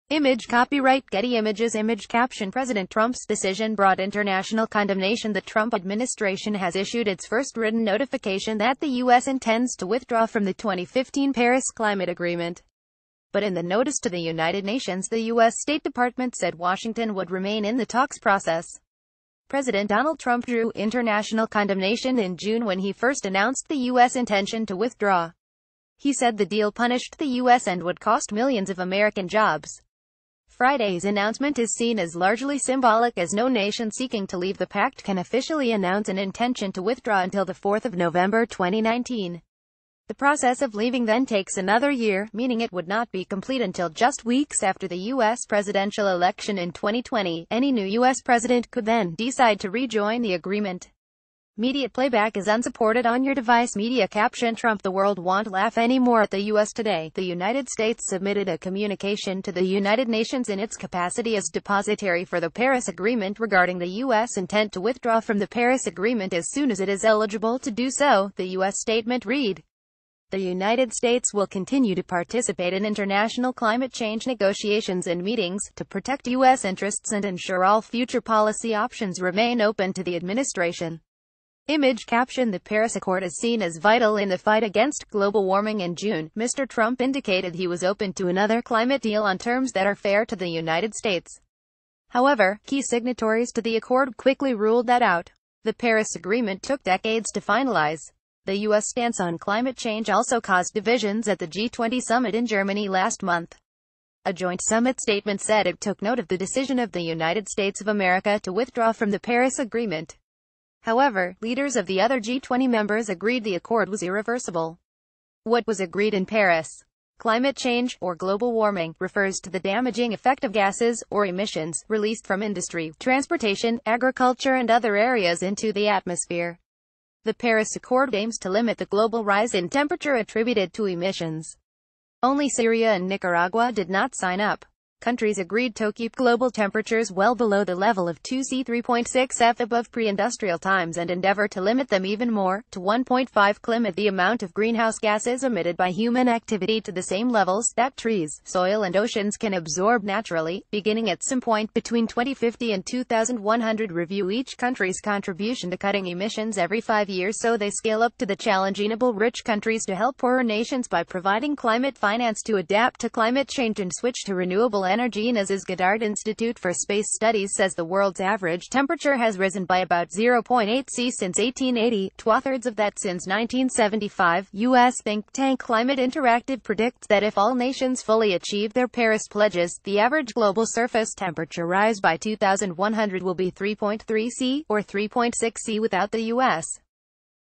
Us notifies un of paris climate deal pullout - bbc news
greenpeace activists display a banner during a rally in front of the us embassy in jakarta on june 7, 2017